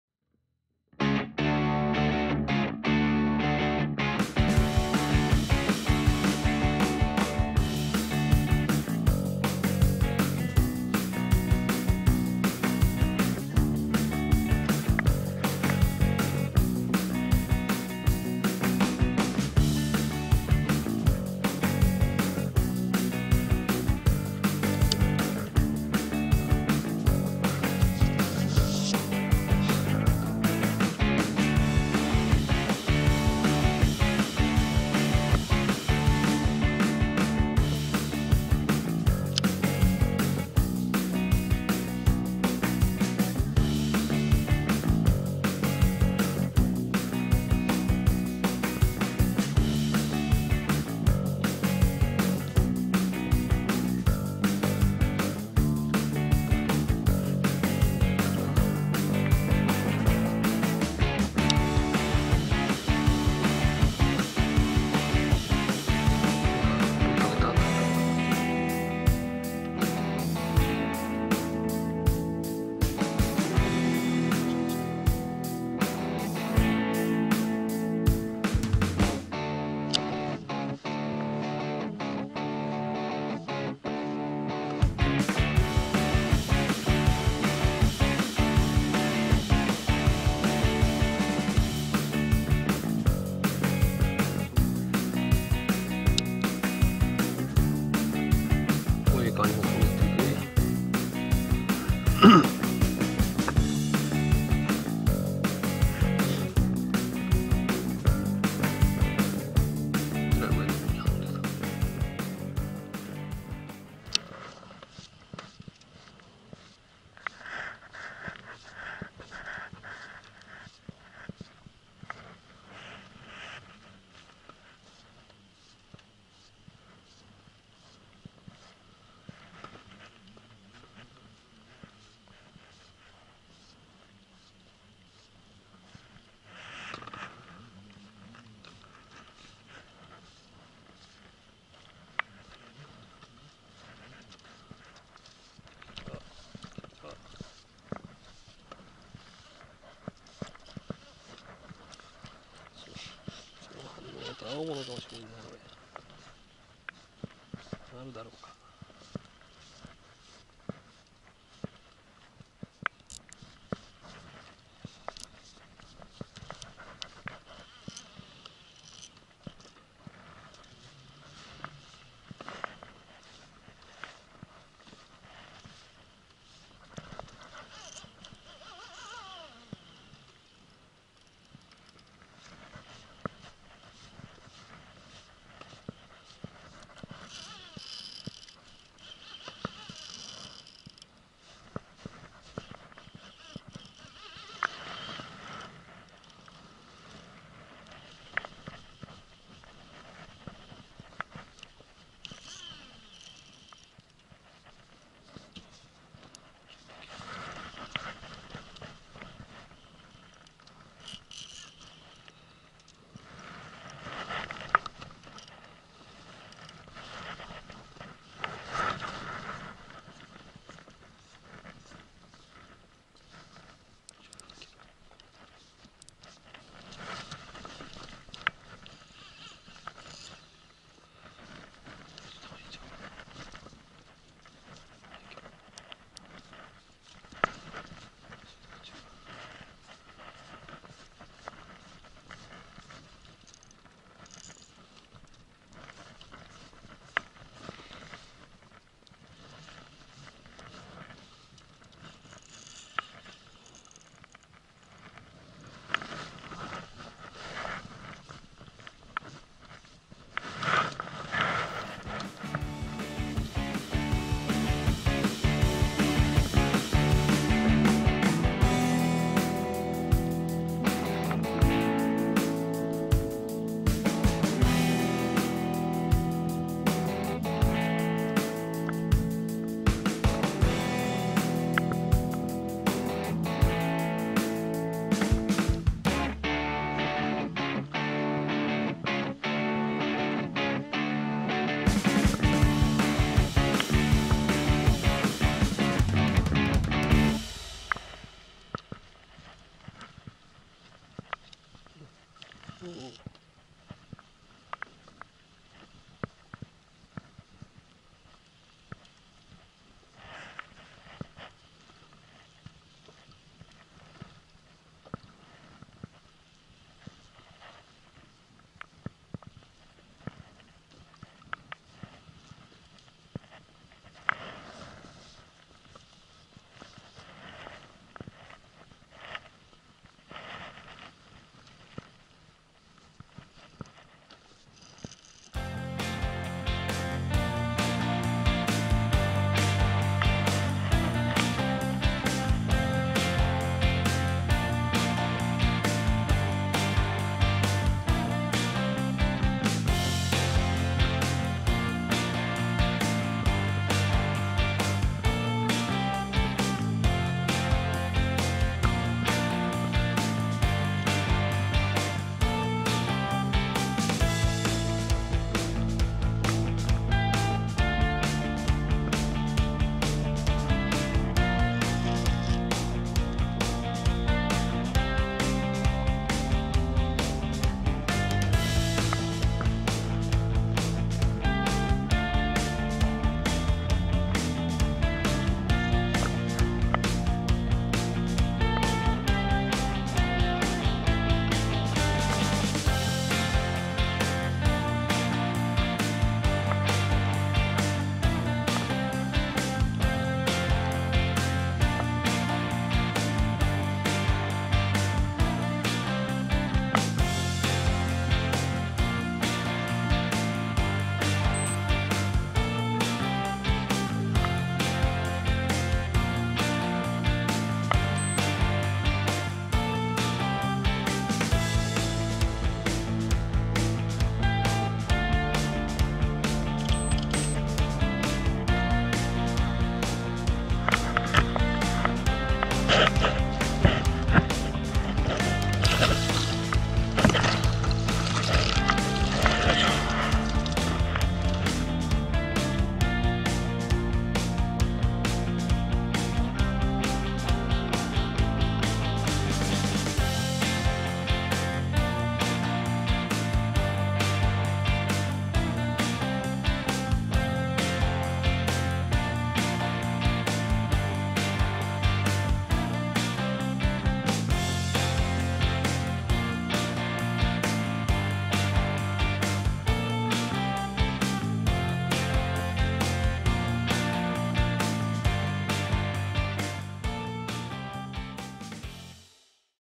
中層の魚探反応にタイラバを落としたら・・・
この日は朝から60真鯛をはじめ・・・
良型の魚を次々キャッチ。

そろそろジギングにシフトしようかと・・・
考えつつタイラバを巻いていると、
魚探に熱い反応が・・・
中層付近を真っ赤に染めます。

青物のはんのうでしょうか？

そのとき、竿先に強烈な前アタリが・・・